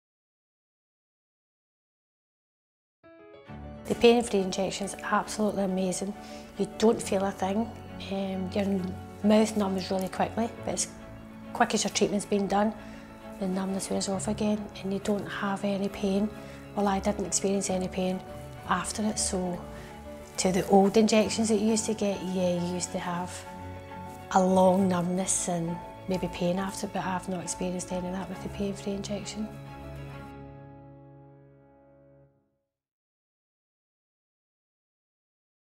Our patient Margaret talks about her experience with our Pain Free Injection System
Our patient Margaret talks about her experience with our Pain Free Injection System. "The pain free injection's absolutely amazing!"